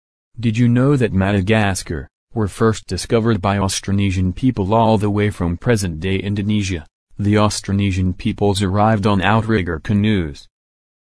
Did you know that MADAGASCAR...
Did you know that Madagascar were first discovered by Austronesian People all the way from present-day Indonesia, the Austronesian Peoples arrived on outrigger canoes.